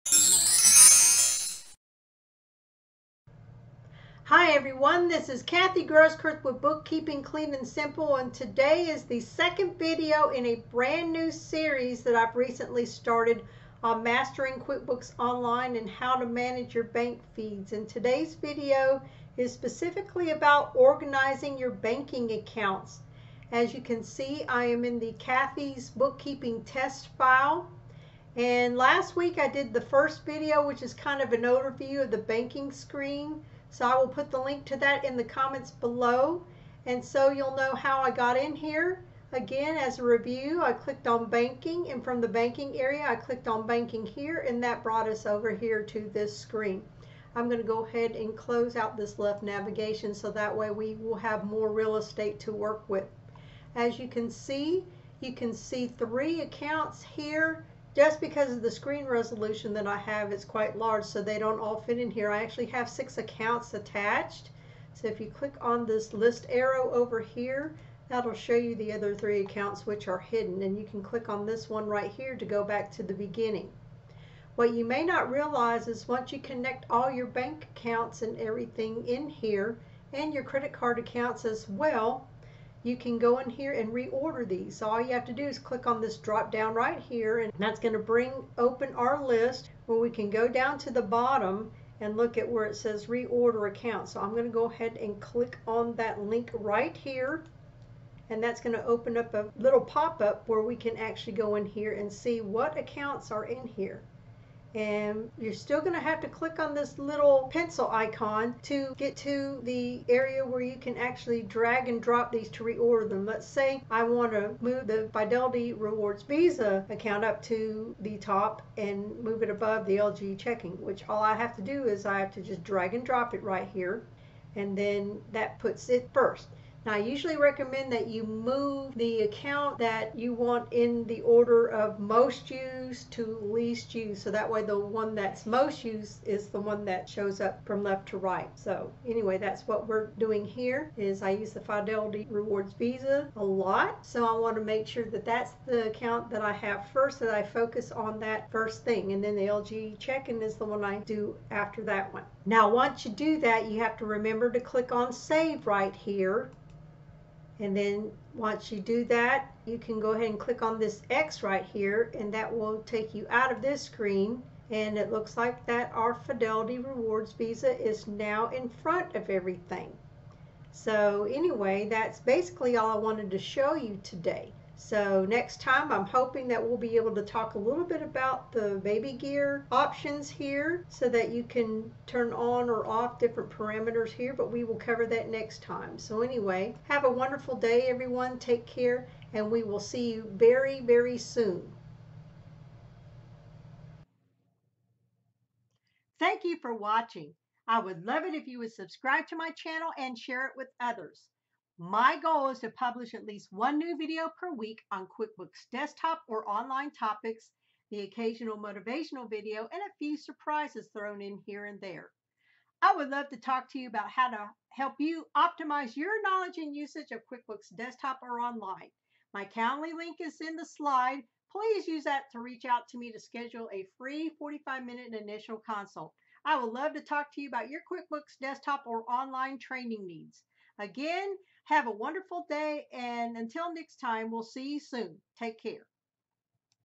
How to Manage Bank Feeds in QuickBooks Online (QBO) - Organizing your Banking Accounts
Today's video is a continuation of a multi-part series on how to manage bank feeds in QuickBooks Online (QBO). Today's video is specifically about organizing your banking accounts in the Banking Center. I recommend reordering the accounts from left to right in order of heaviest usage to least used.

00:00 - Introduction
00:37 - How to access the Banking Center
00:57 - Scrolling to see all attached accounts
01:24 - Reorder accounts menu access
01:34 - Click on link to open editing window
01:45 - Click pencil to reorder
01:53 - We will move Fidelity to first spot
02:06 - My recommendations for reordering
02:32 - Don't forget to save your changes!
02:37 - Click on the X to return to Banking
02:54 - Wrap up and what's next
03:14 - Outtro and how to contact me